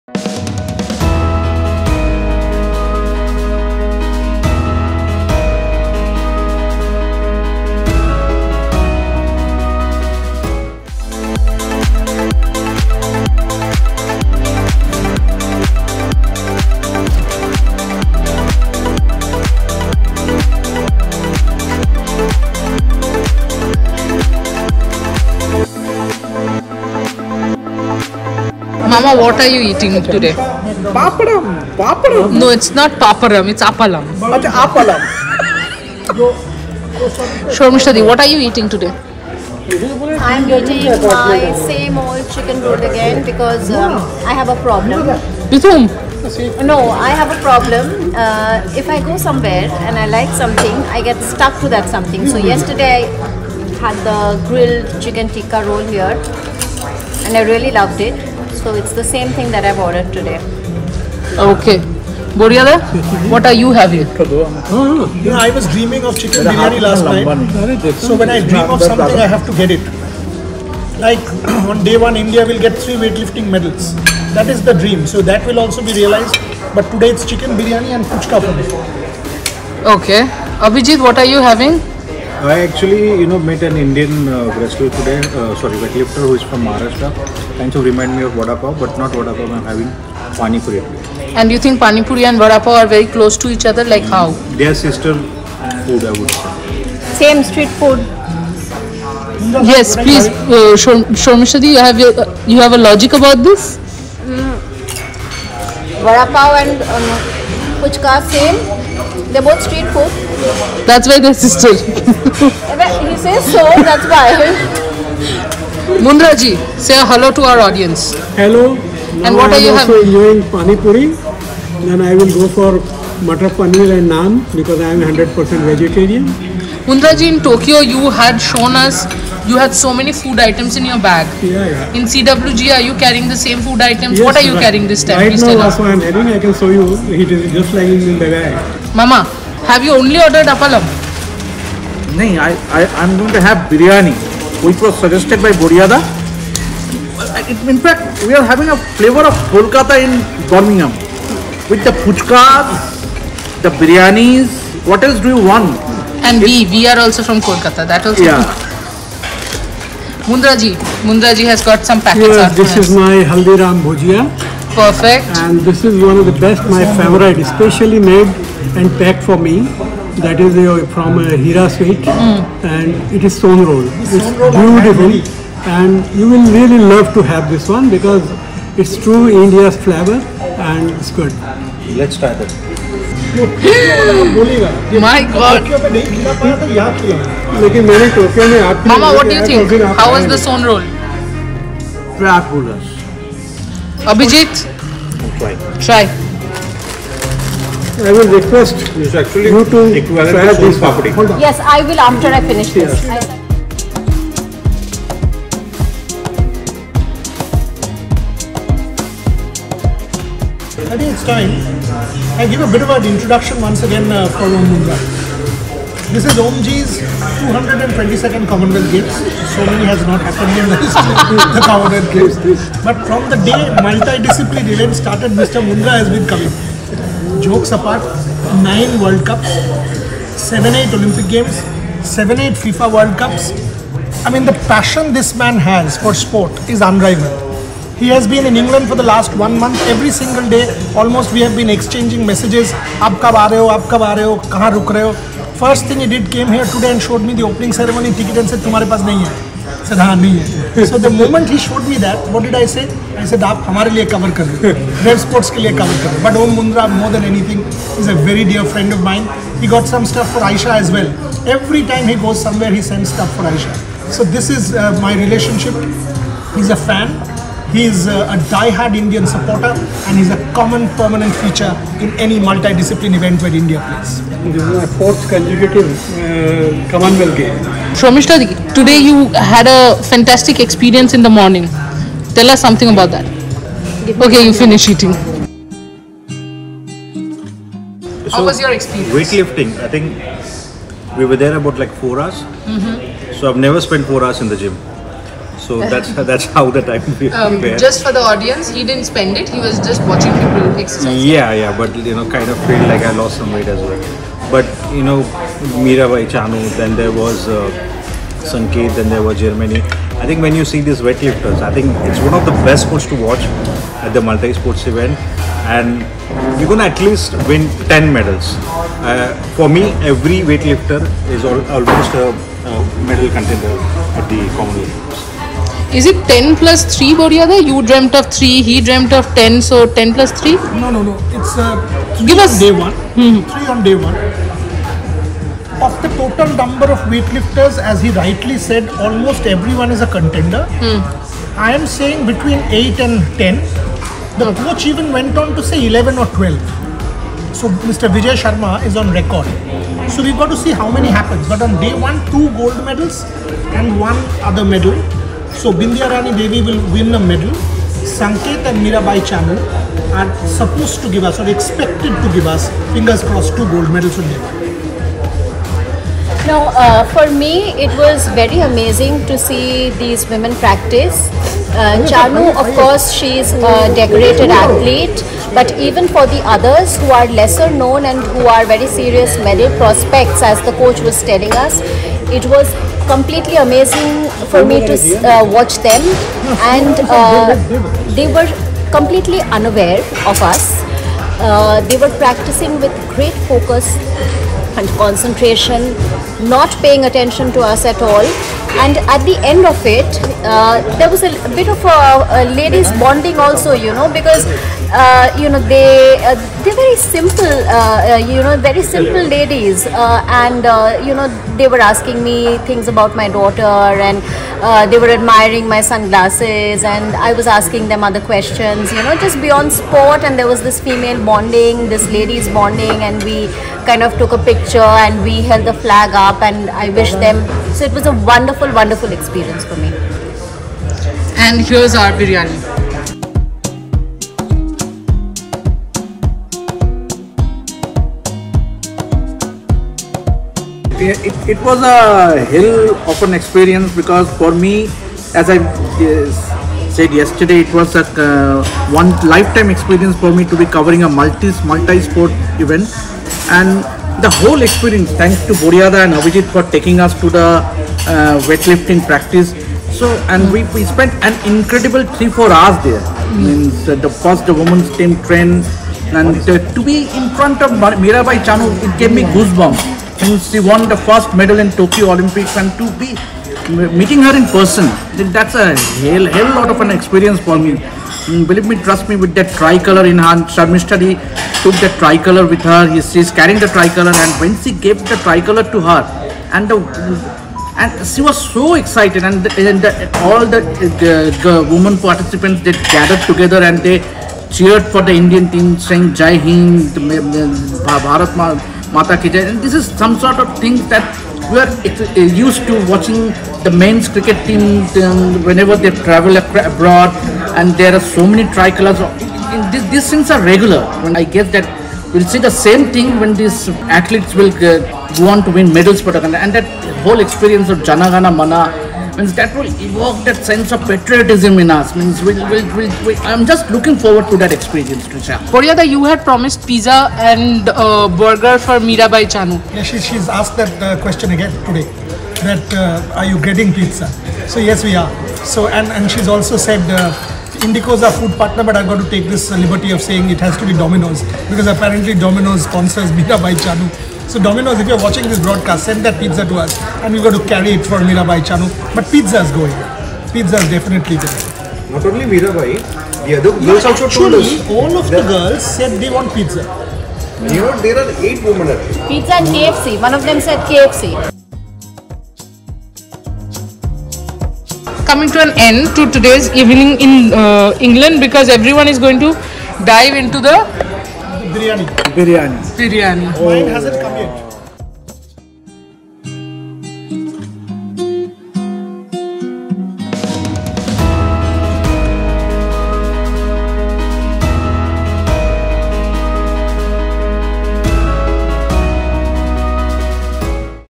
Evenings in England EP02 || CWG2022
The Games start tomorrow ✅
Team @RevSportz will bring every aspect of it and add flavour with @Indico_street in this special segment Evenings in England, presented by @OfficialFanatic & @Just_My_Roots.